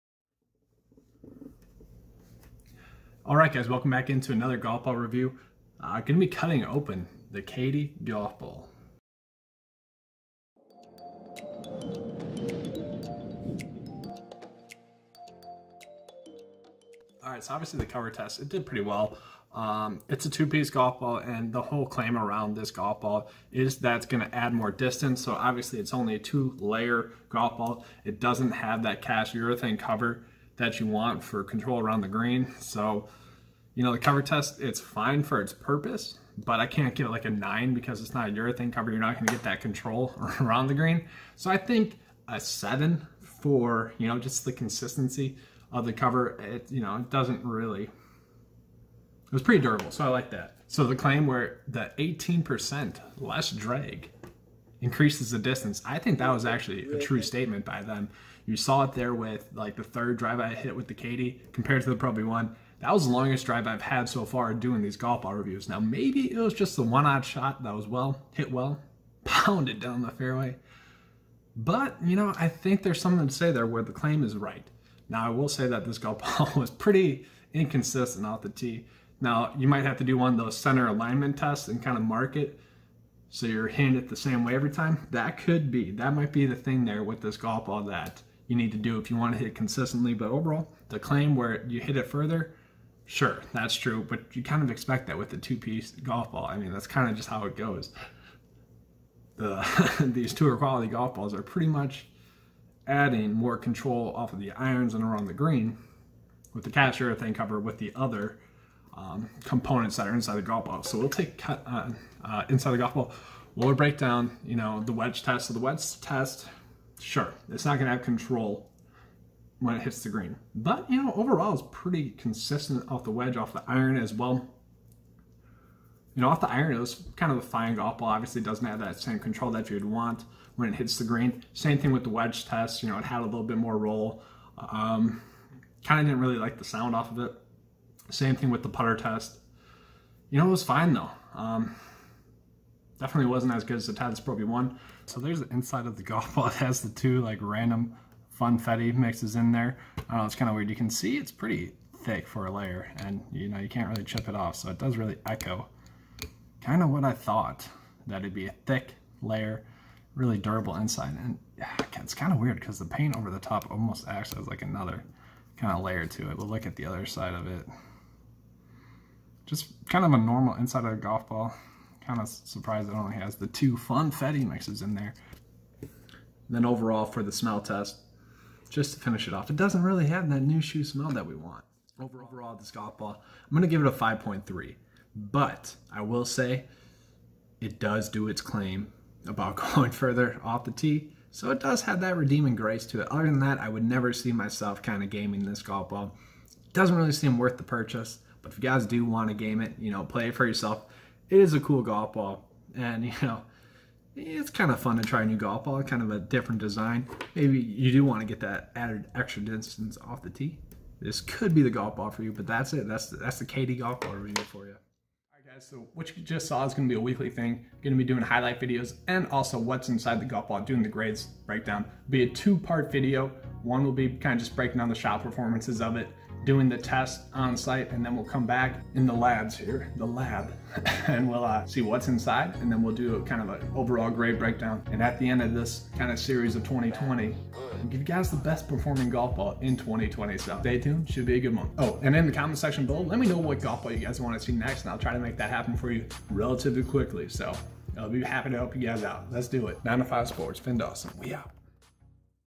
KAEDE PRO - Different Than I Expected...
The KAEDE PRO was a fun golf ball to play. But it claims that its floral design will decrease the drag by 18% thus allowing the golf ball to go further.